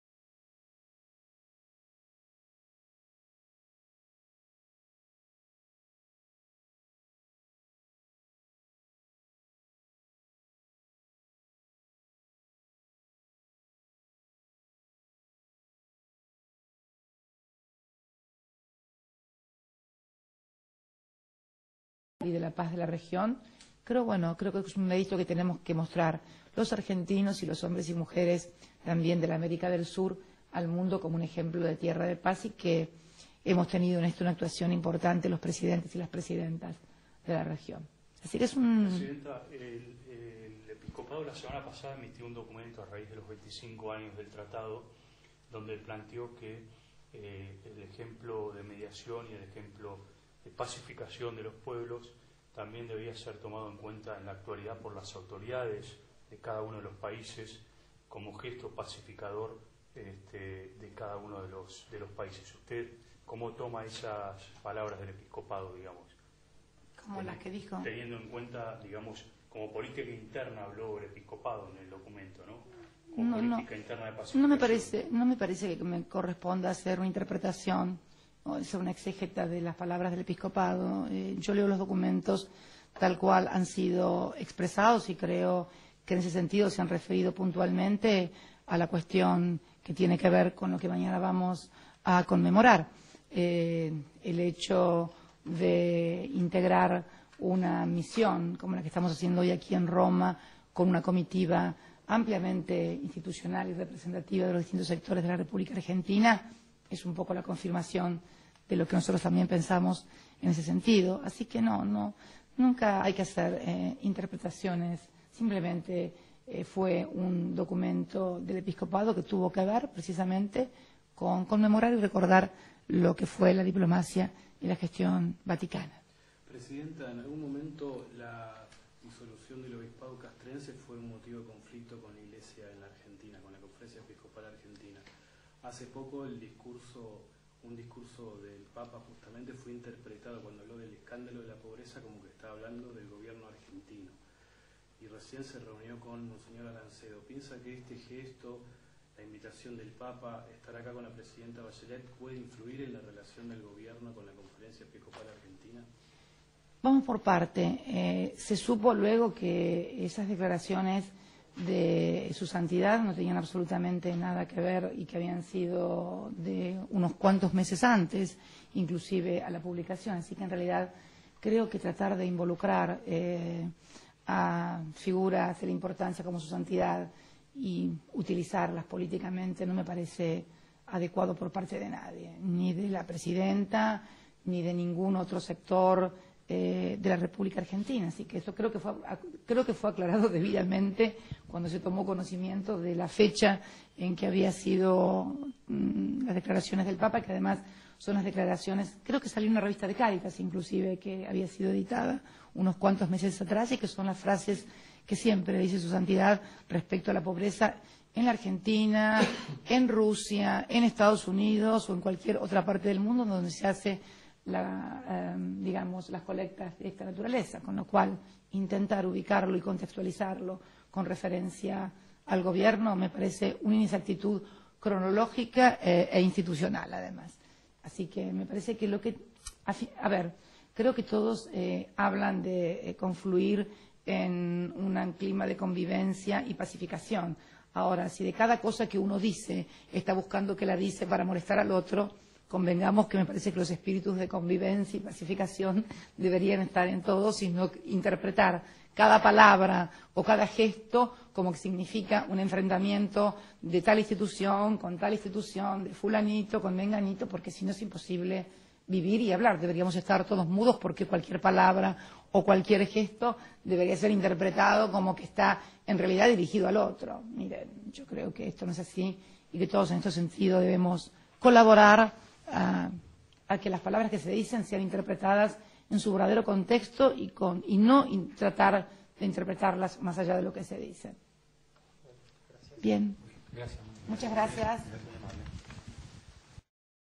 27 de Noviembre 2009 2 La Presidenta llegó a Roma Cristina conferencia.wmv
La Presidenta Cristina Fernandez
llegó a Roma  conferencia
de prensa
 27 de Noviembre 2009
parte 2 de 2
Editó : Jeremias